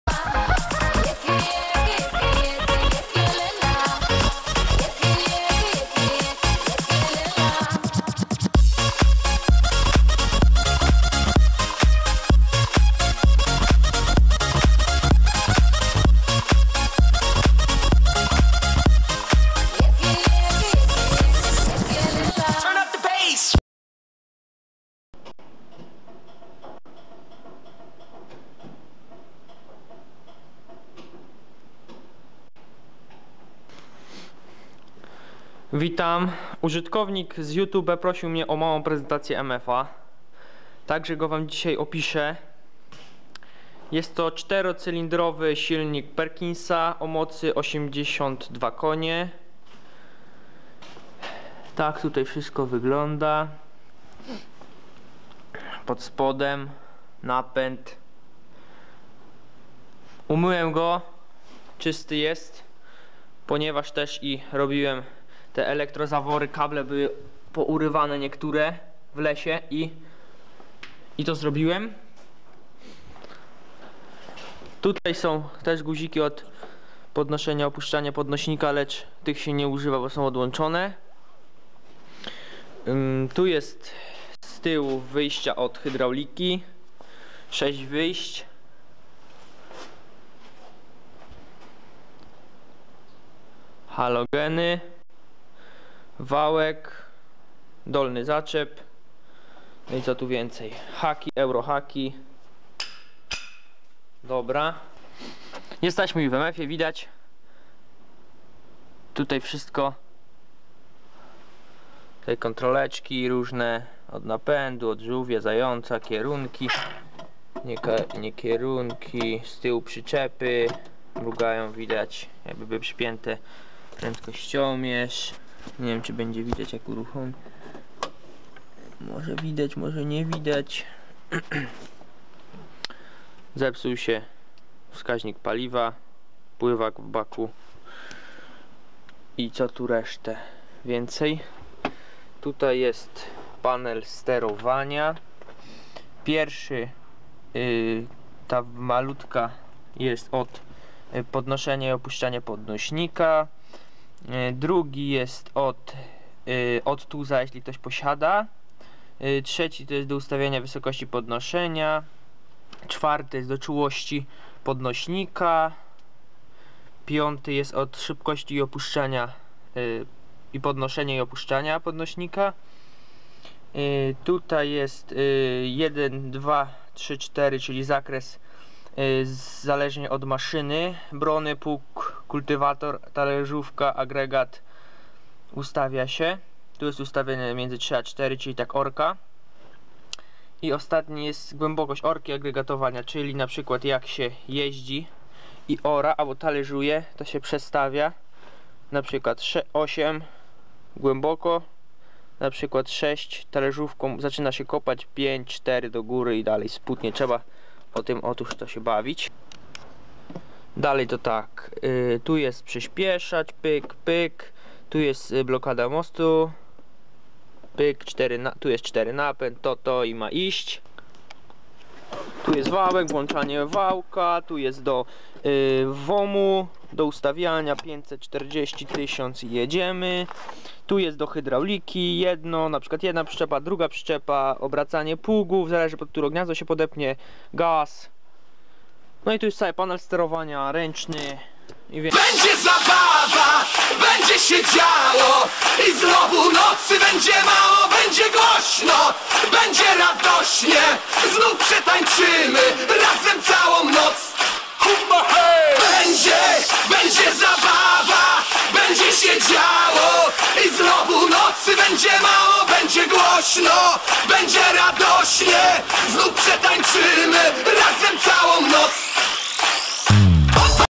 ●̮̮̃•̃ Massey Ferguson czyli Prezentacja Ferdka ●̮̮̃•̃
●̮̮̃•̃ Siemasz !●̮̮̃•̃

●̮̮̃•̃ Dziś pokazuje wam film na prośbe użytkownika : DJ POWER .

●̮̮̃•̃ Mała prezentacja co gdzie i jak .

●̮̮̃•̃ Już niebawem film ze Stodoła na Oborę :D ( już mamy ją wysprzątaną :)

●̮̮̃•̃ Oglądajcie do końca warto :)